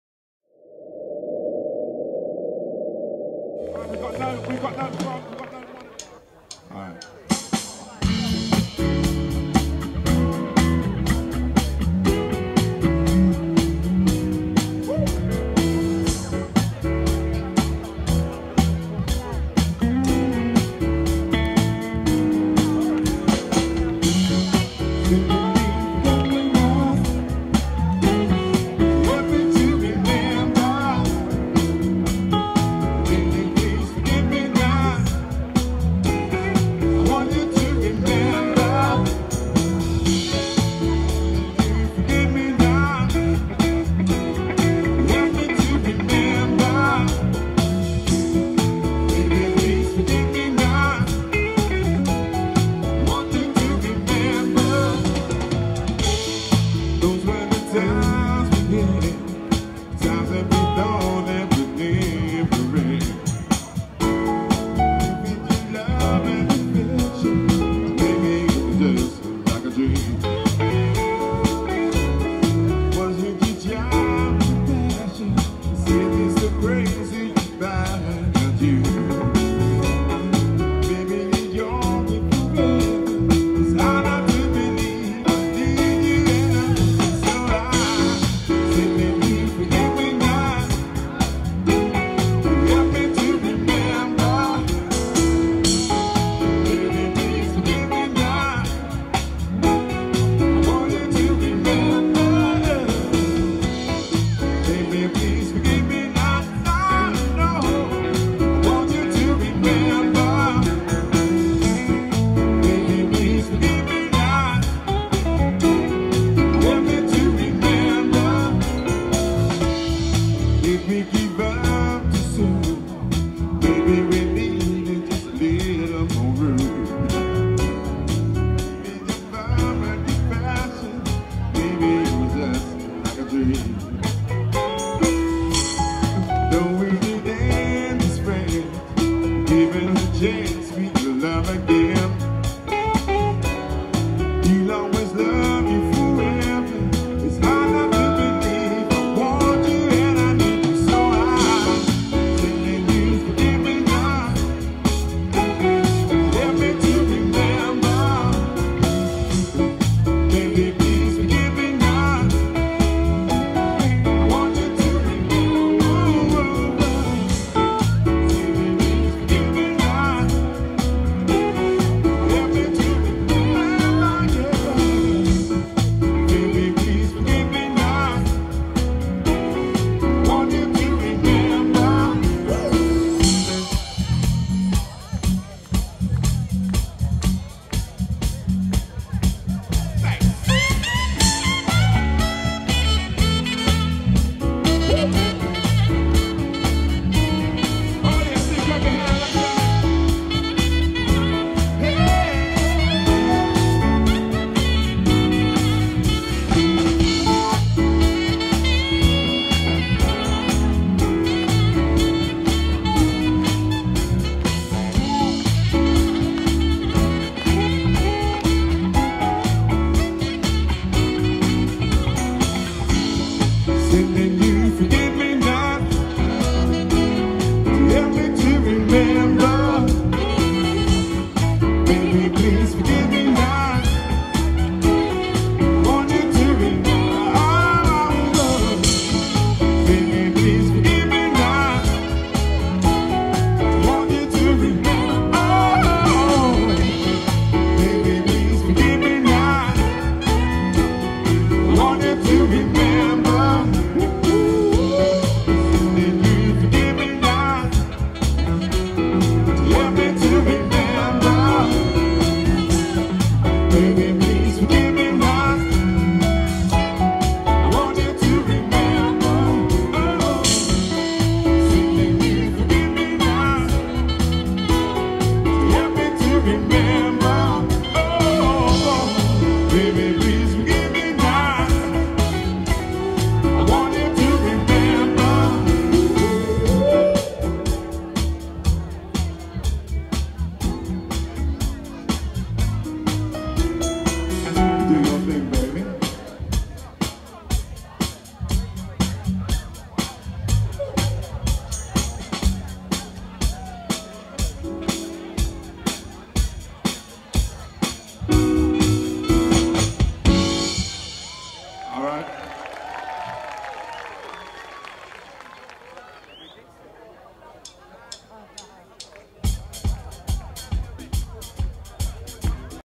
IRIE J - Live at Ingatestone Hall [Live Performance]
[BLUE CANARY FILMS]
IRIE J - Live at Ingatestone Hall

Filmed & Edited by Blue Canary Films

To get in contact with Irie J, check out his official